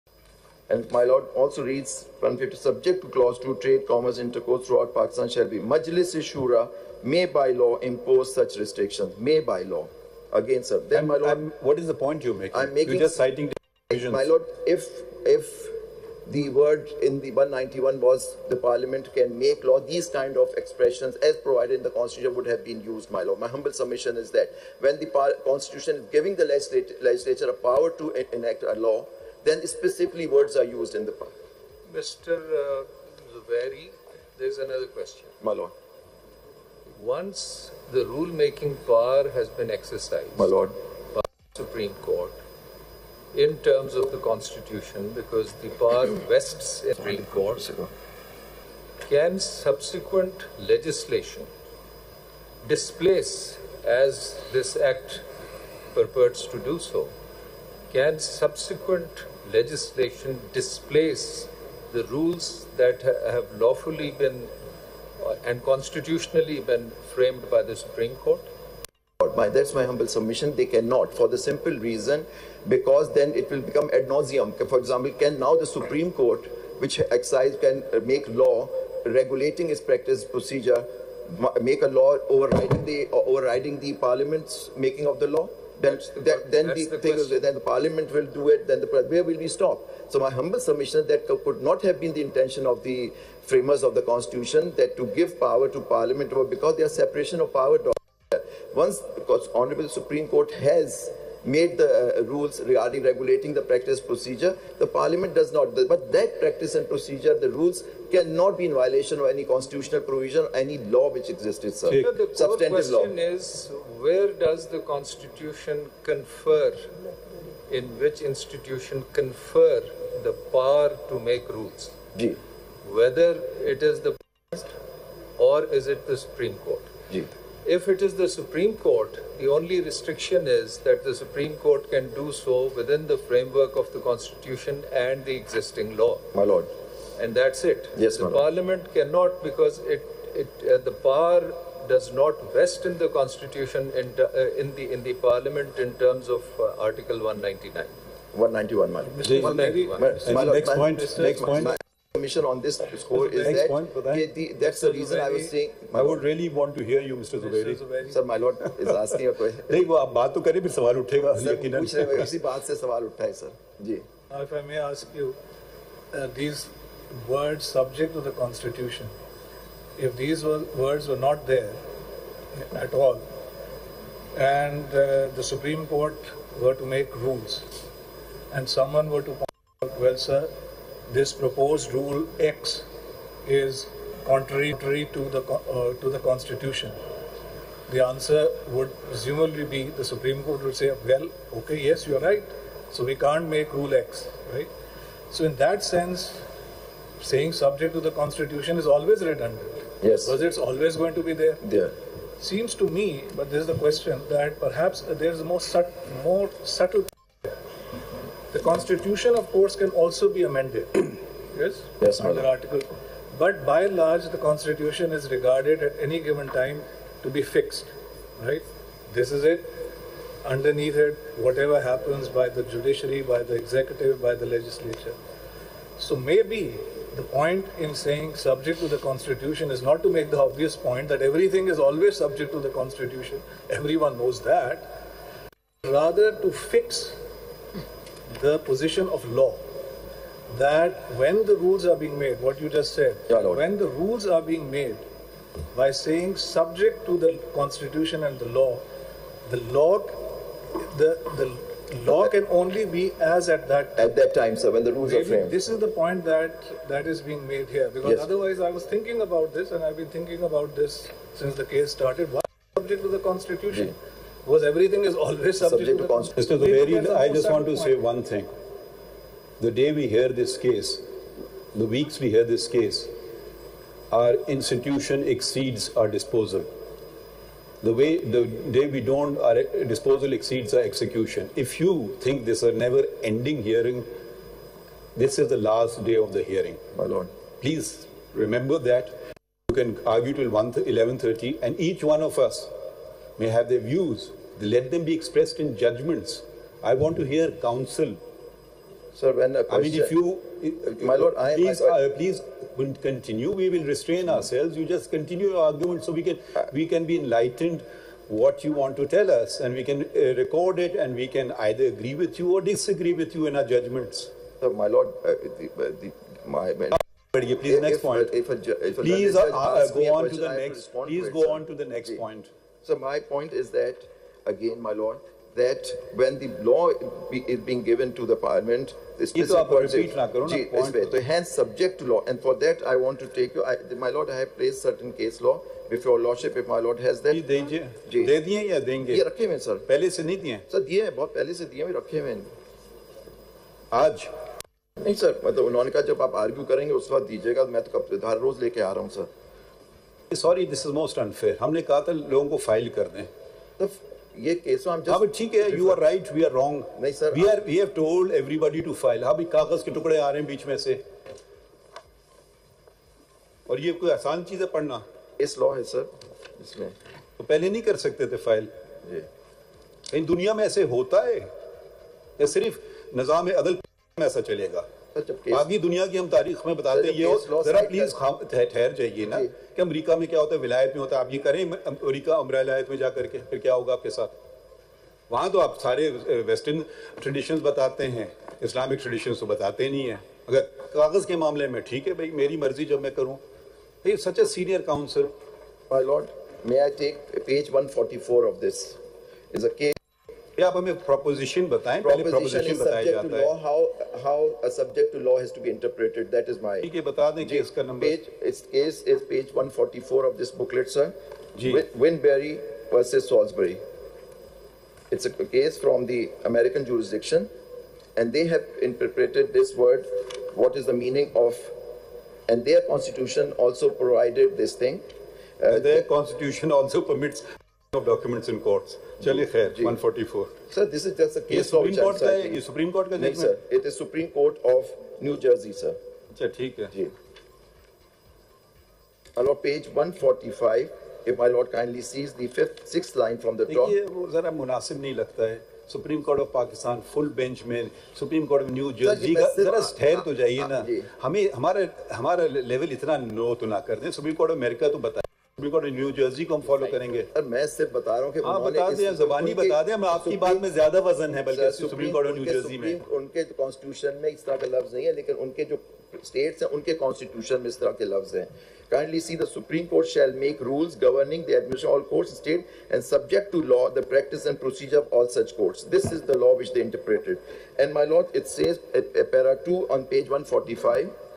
Hearing of Supreme Court Practice and Procedure Act | Dunya News
The full court bench is headed by Chief Justice of Pakistan (CJP) Qazi Faez Isa and includes Justice Sardar Tariq Masood, Justice Ijazul Ahsan, Justice Syed Mansoor Ali Shah, Justice Munib Akhtar, Justice Yahya Afridi, Justice Aminuddin Khan, Justice Sayyed Mazahar Ali Akbar Naqvi, Justice Jamal Khan Mandokhel, Justice Muhammad Ali Mazhar, Justice Ayesha A. Malik, Justice Athar Minallah, Justice Syed Hasan Azhar Rizvi, Justice Shahid Waheed and Justice Musarrat Hilali.

Addressing the lawyers gathered, the CJP urged them to talk about points that were relevant to the case, adding that the bench wants to try to conclude the case today.

Justice Isa said many cases are already pending in the SC and the court cannot spend too much time on one case.